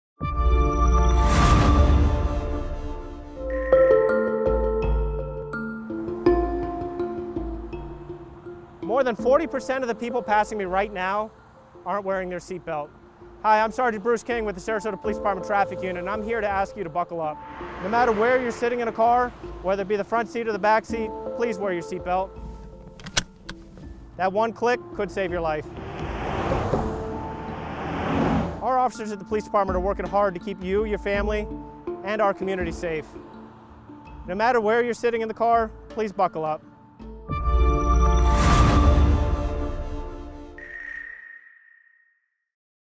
Sarasota Police: Always Buckle Up
The Sarasota Police Department wants to remind you no matter where you're sitting in a car, always buckle up.  Click it or ticket.